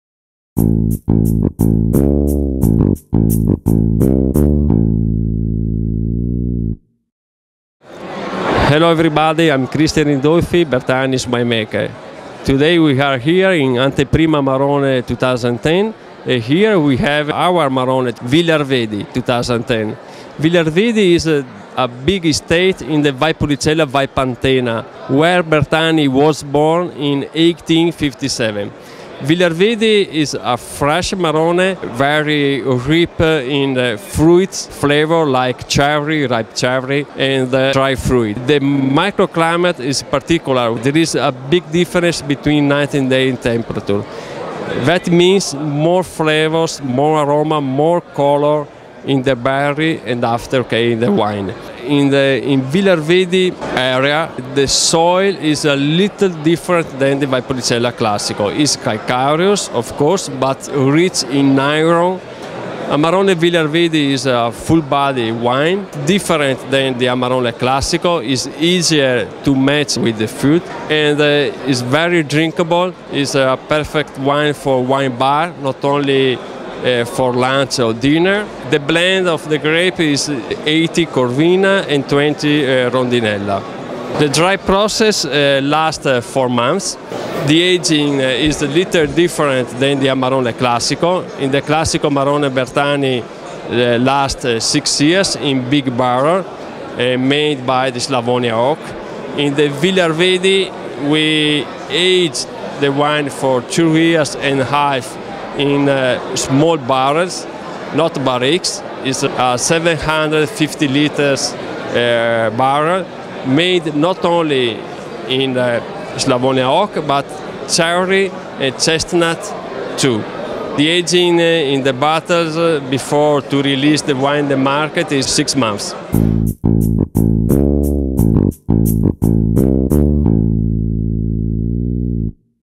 "Amarone" Tv - Bertani Amarone Villa Arvedi - "Italian Wine" by "Confraternita Amarone"
At "Anteprima Amarone 2010", Christian Ridolfi, introduces Amarone Bertani "Villa Arvedi"  vintage 2010, the first under the DOCG appellation.
"Amarone TV" is a production by "Stile Italia" in collaboration with "Confraternita dell'Amarone. Videos are realized and directed by Danilo della Mura